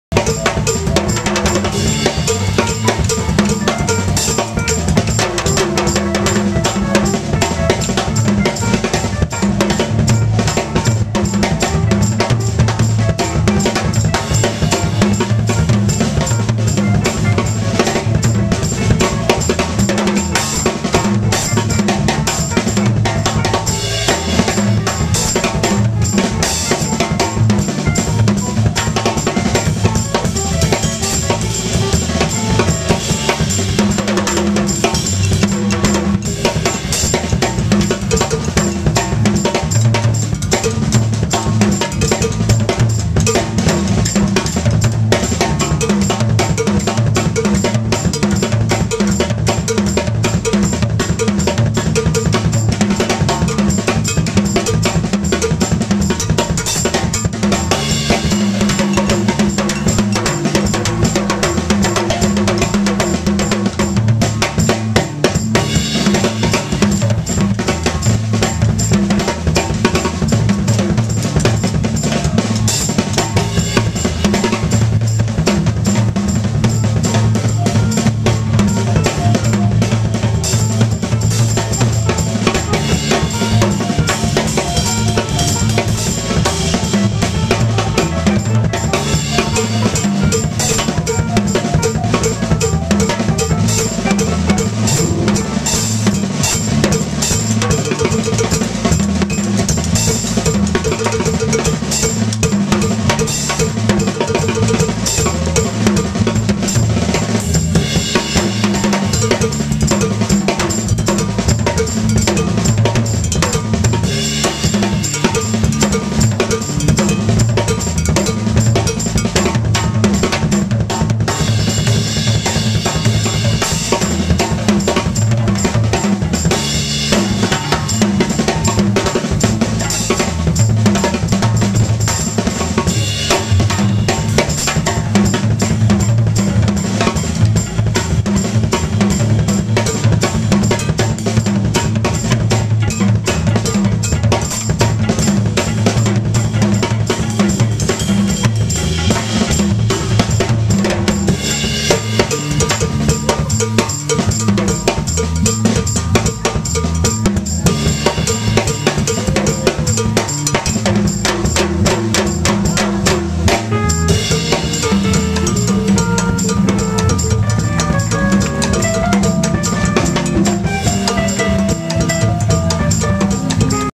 Drum solo over Montuno
Joe Truglio soloing over a montuno @ "Time" bar in Philly